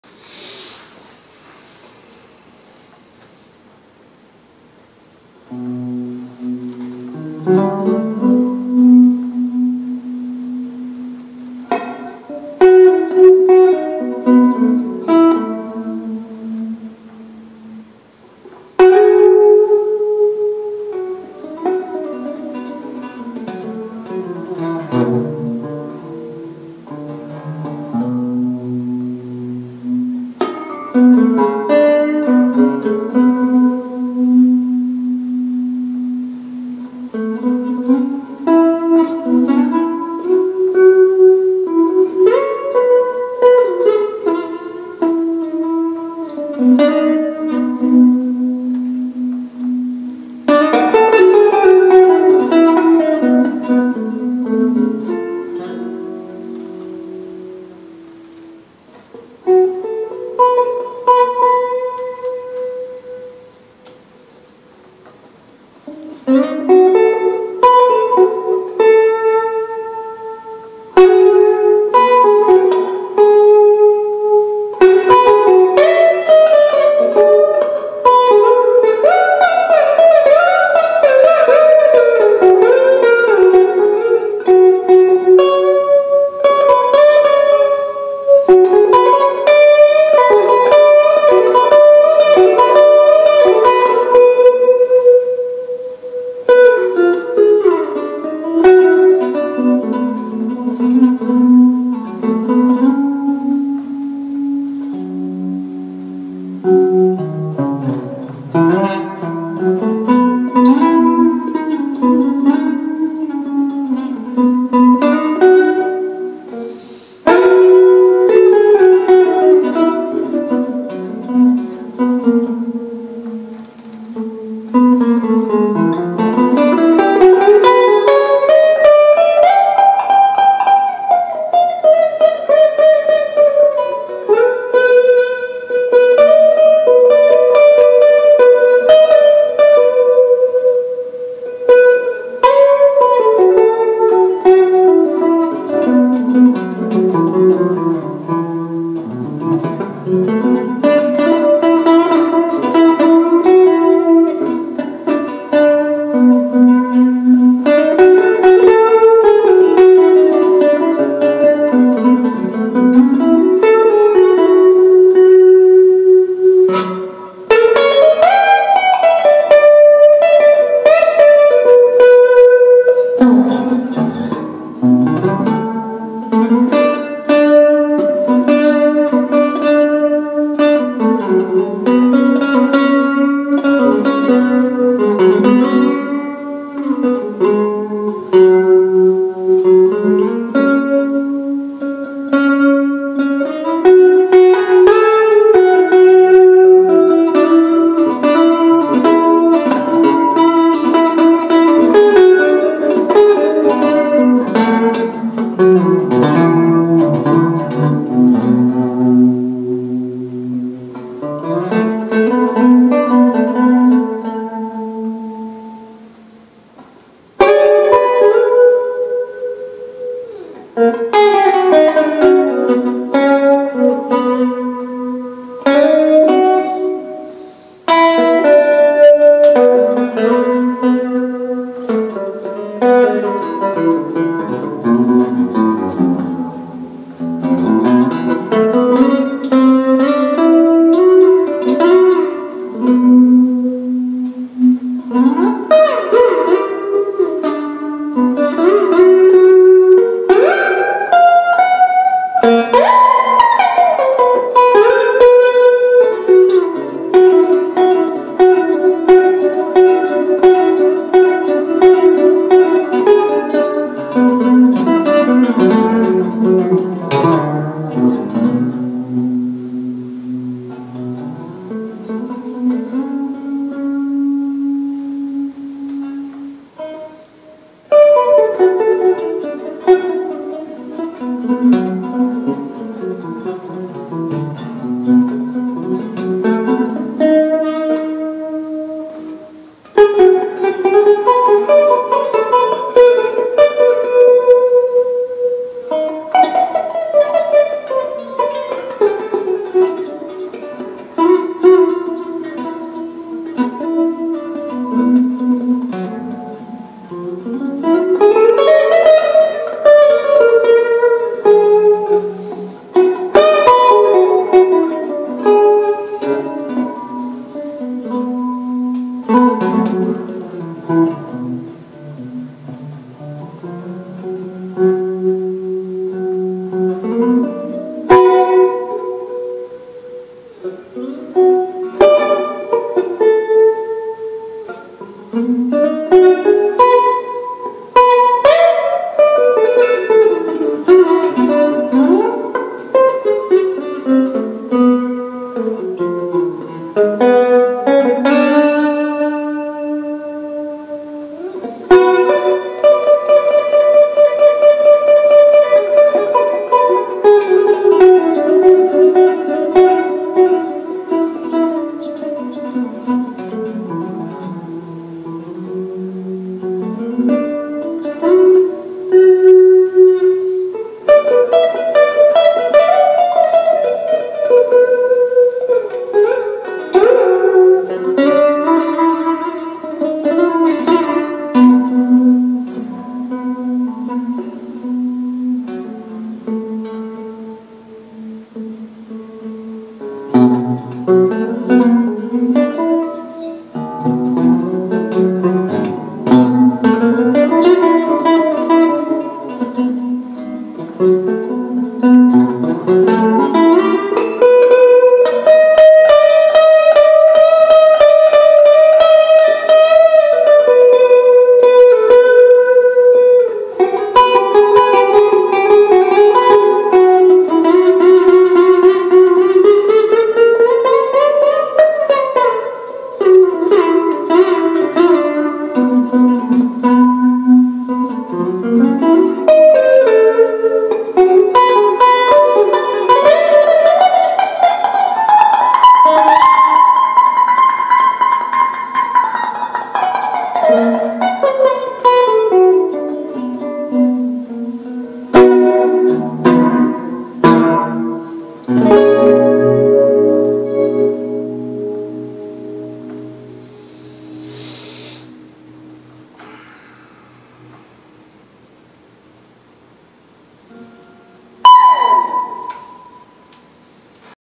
The Blues in Black and Blurs
Freeform blues improv. Sorry about the focus issues...blame the cell cam and its spastic autofocus.

This is my home-built Jazzbo Hepcaster with a Lollar El Rayo pickup. Going into a Hardwire Supernatural Ambient Verb (probably on the plate setting) and into a 5F1-style tweed L'il Punk amp (a champ clone with a 12" speaker) built by Modern Sound Amps. All killer. No filler.